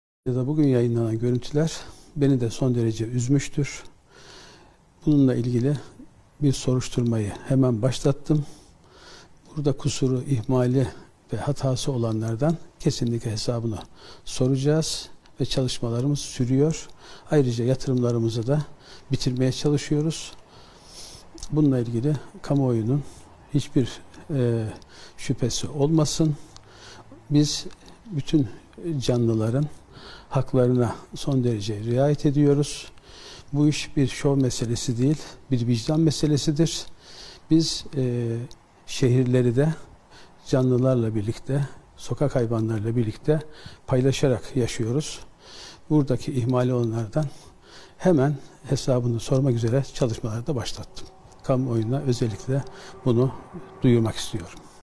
Hilmi Güler'den Hayvan Barınağı Açıklaması
Ordu Büyükşehir Belediye Başkanı Dr. Mehmet Hilmi Güler, sosyal medyada yayınlanan Ordu’daki hayvan barınaklarının durumu ile ilgili açıklama yaptı.

Başkan Güler, “Sosyal medyada bugün yayınlanan görüntüler beni son derece üzmüştür. Yaşanan olayda hatası, kusuru ve ihmali olan tüm yetkililer hakkında soruşturma başlattım. Herkes gereken cezayı alacak. Bu iş bir şov meselesi değil, vicdan meselesidir. Kamuoyuna saygılarımla duyurulur” dedi.